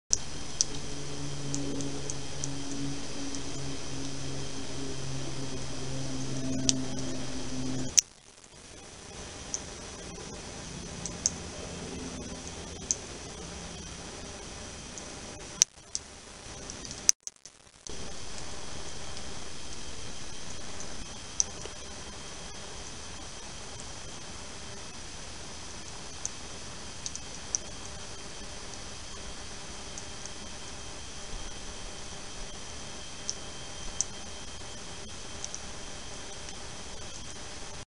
Scotch or Scots Pine popping
I recorded this in the Arboretum at the University of Guelph, Ontario, Canada - The Scotch Pines (that was the name they used, Scots Pine is the common form)  were opening their cones in the spring sunshine around the end of March, and making a very loud popping noise that could be heard from 30 feet away. Unfortunately it's right next to an airport. All photos wikimedia commons. Sound recorded some time in the 1990s.